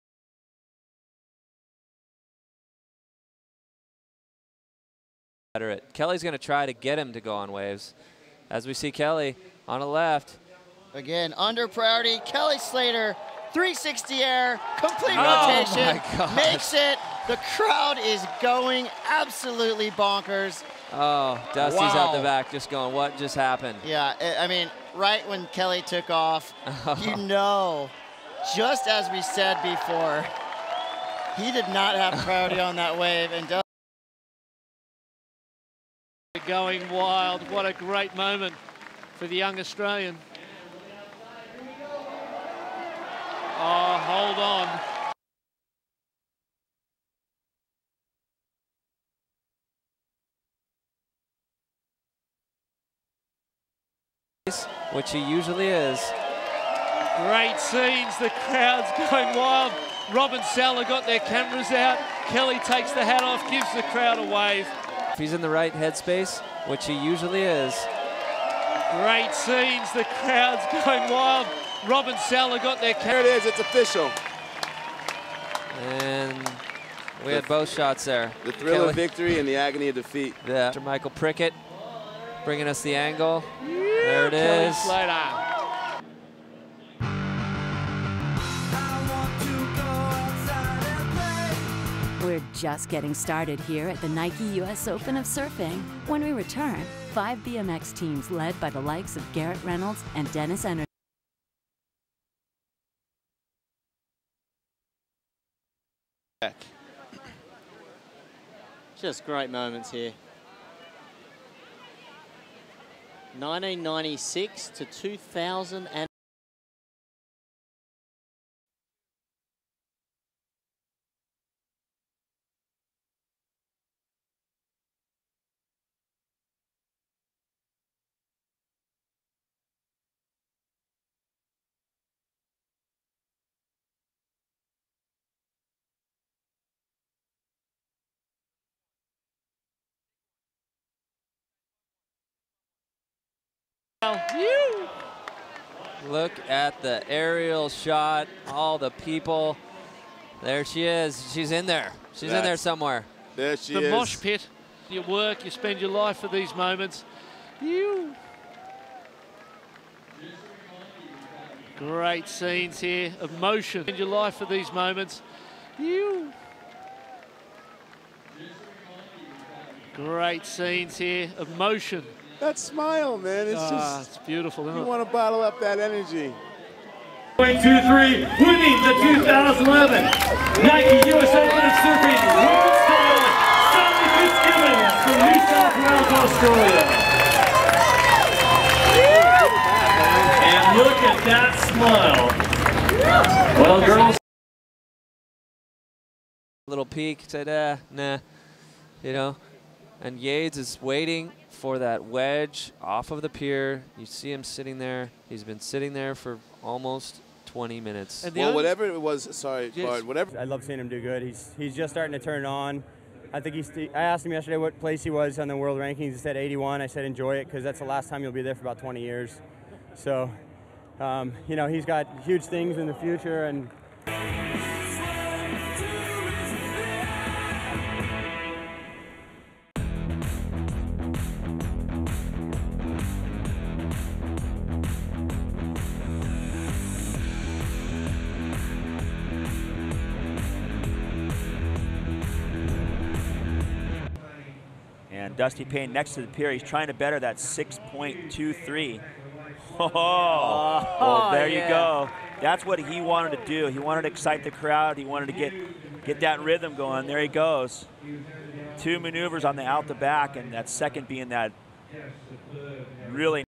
US Open of Surfing Final Surf Highlights
Kelly Slater vs Yadin Nicol.  Kelly Slater 2011 US Open of Surfing Champion. Semi-finals Kolohe Andino, Dusty Paine.  Video courtesy Hurley/Nike